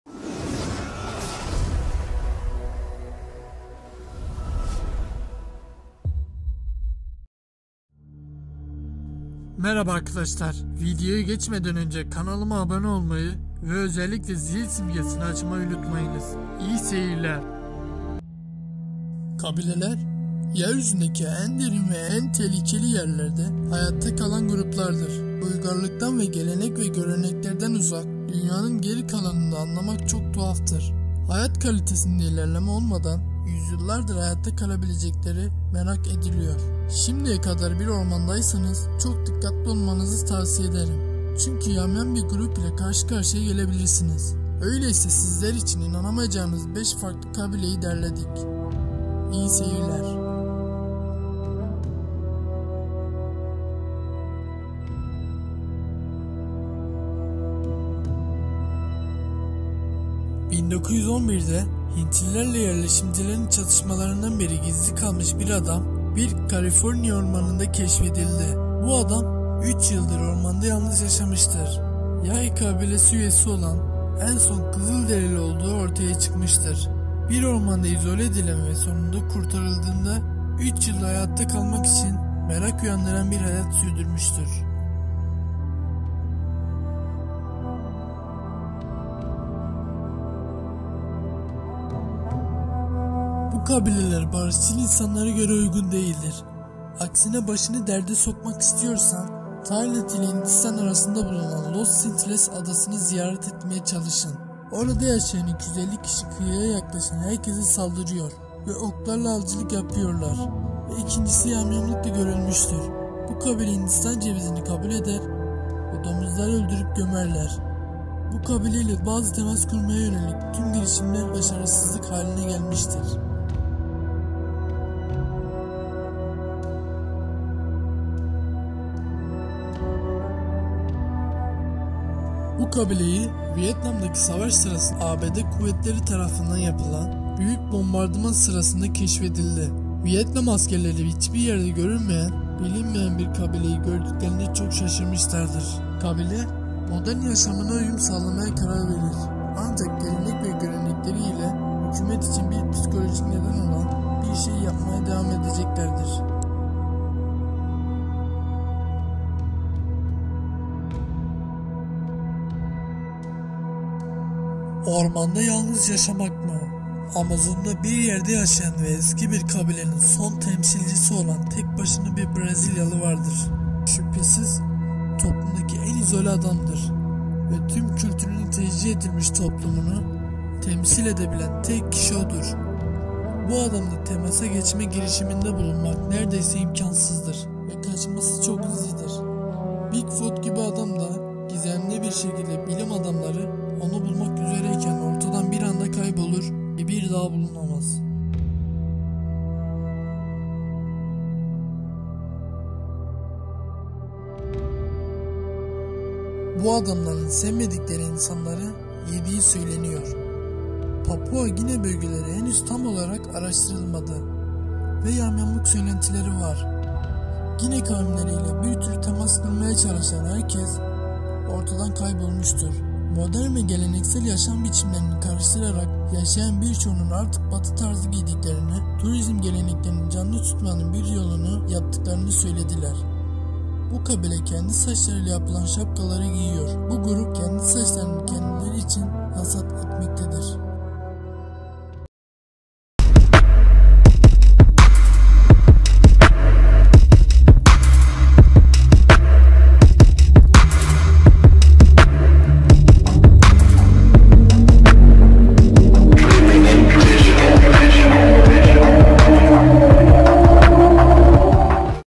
Varlığına inanamayacağınız 5 Sıradışı Kabile
Varlığına inanamayacağınız 5 Sıradışı Kabile...Videoyu Beğendiyseniz Lütfen Like Atmayı Unutmayınız. İYİ SEYİRLER...

LİNKLERE BAKALIM LÜTFEN .

Genellikle Kullandığım Kaynak siteler

► faturetimeline.net
► humanknowledge.net

Diğer Video Listeleri: Lütfen Birini izle!